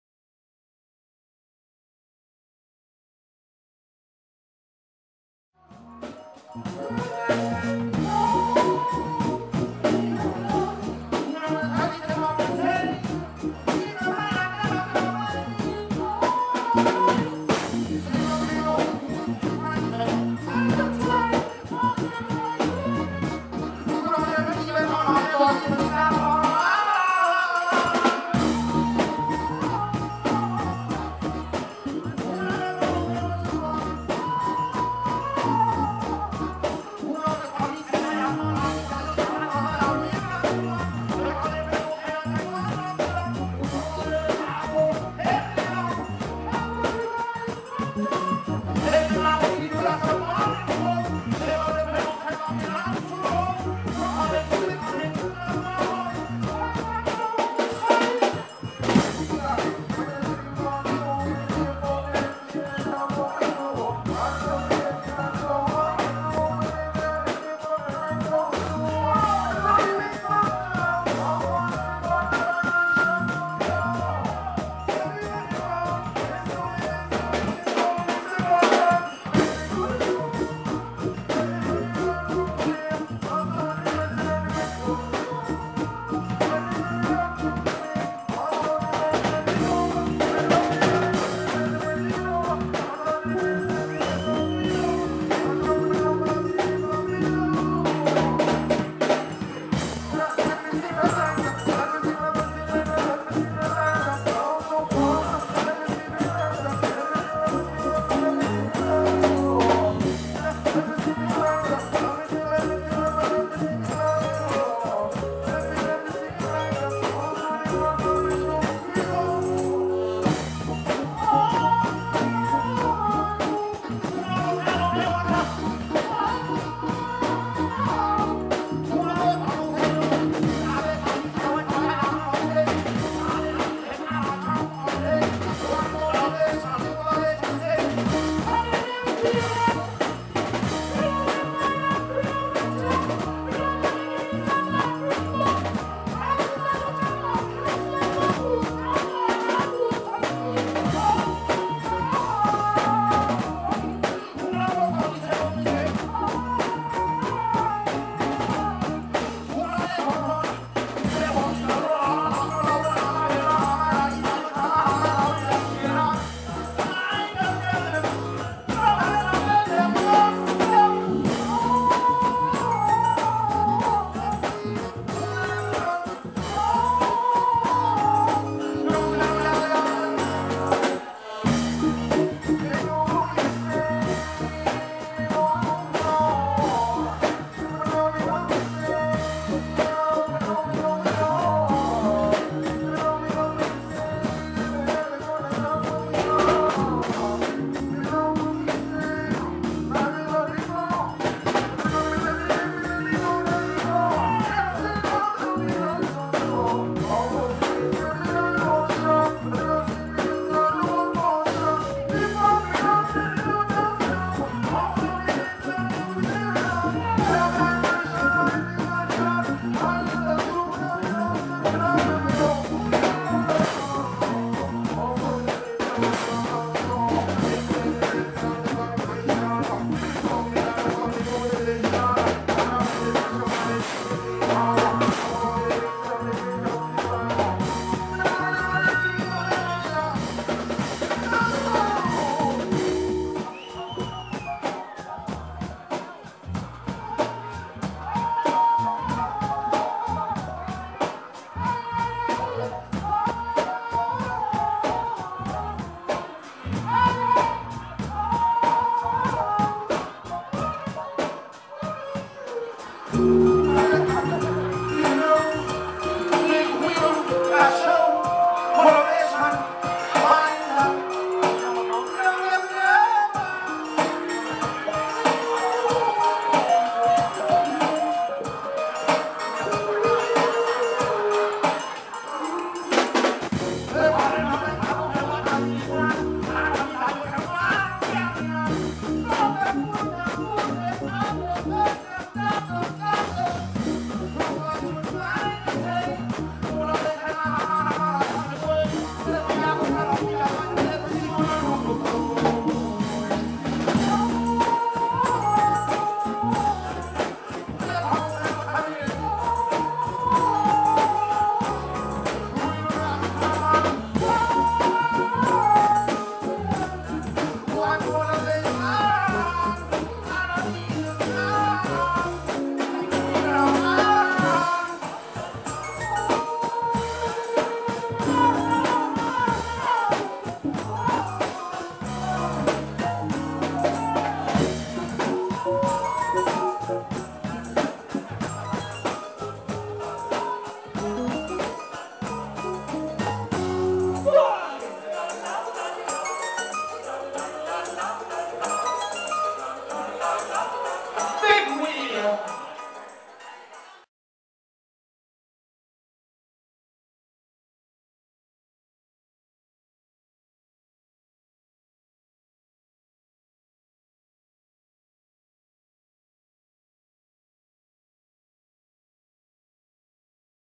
SpellBinder and Etef (DJ Cavem) Performing @ Appaloosa Bar and Grill
This is SpellBinder in an autotune version.